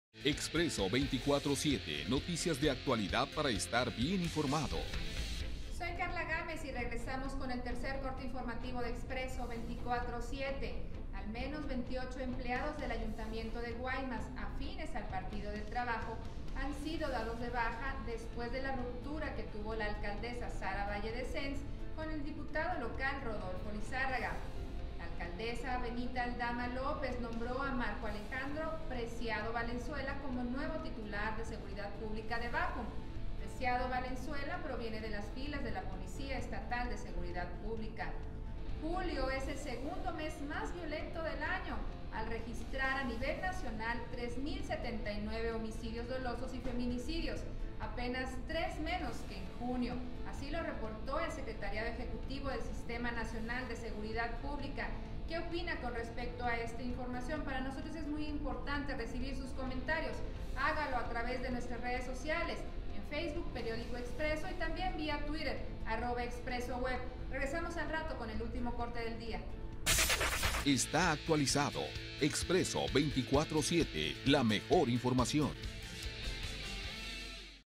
Ayuntamiento de Guaymas da de baja a empleados y en Bácum ya hay nuevo comandante de Policía
Ayuntamiento de Guaymas da de baja a empleados y en Bácum ya hay nuevo comandante de Policía... Tercer resumen informativo de Expreso24_7 con Karla Gámez Ávila en Expreso